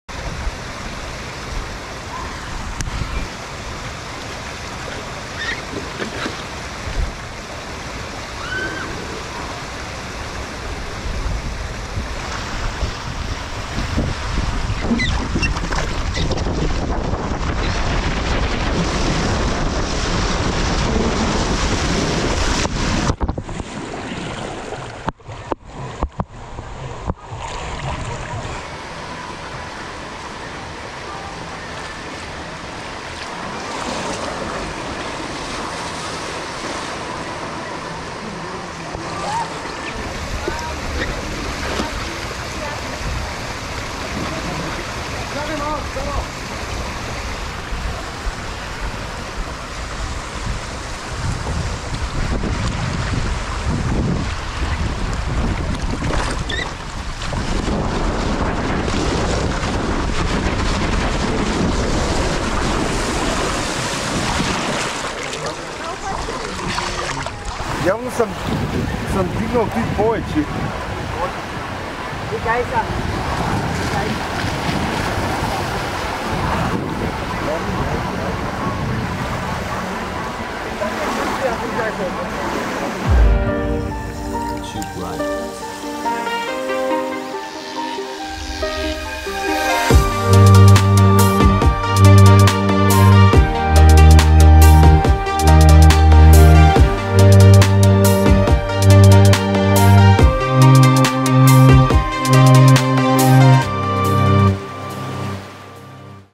Multilane Slide at Aquapark Neptun | Competition Water Slide
Take a ride on the Multilane Slide at Aquapark Neptun in Sozopol, Bulgaria. The four-lane racer water slide is fitted with a bump hill, making for some airtime. Unlike the majority of other mat racer water slides, this one does not feature shallow runout lanes but a deep pool instead. Surprisingly, the landing is still smooth when lifting up your mat.

Type: Multilane Slide (4 Lanes)
Manufactured by: Arihant
Year of installation: 2017